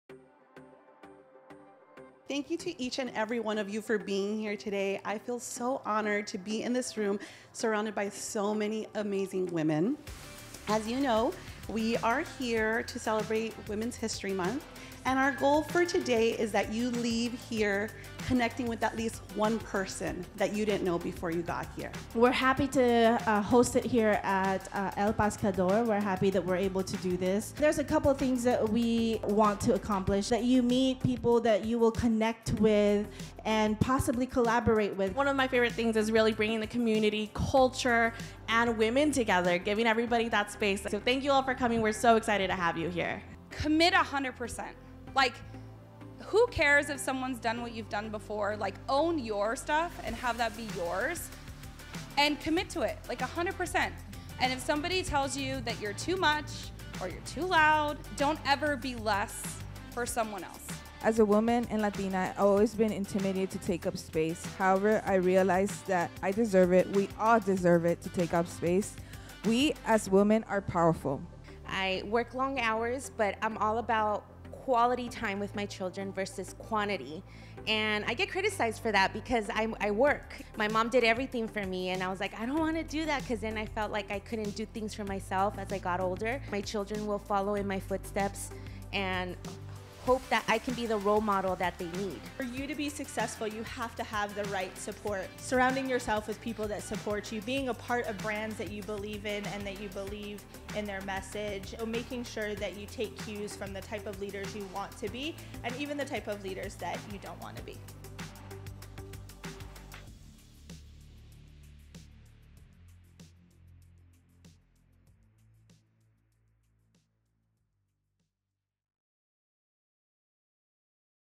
Plaza West Covina's Dreamers and Doers Breakfast Club
March is Women's History Month and Plaza West Covina partnered with Loren from Alvares Adventures to host a breakfast event inviting some of the local digital creators to celebrate!

Thank you to El Pescador for accommodating our breakfast and to Sephora at Plaza West Covina, Macy's Beauty Counter, and Lazy Dog for providing goodies for all of our guests.

Thank you to all of our speakers who took the time to impart inspiring messages about resilience and boldness.

Speakers:
Kristen Carr, Hosts Disney events (Event Creator), a Disney News Podcast and Writer

Brenda Barrios, Talented Multidisciplinary fine artist at UCLA who’s exhibited her work in NY and London

Claudia Uballez 2 Time Emmy Award-winning producer and talent coordinator

Liz Desilva VP of Creative at Loungefly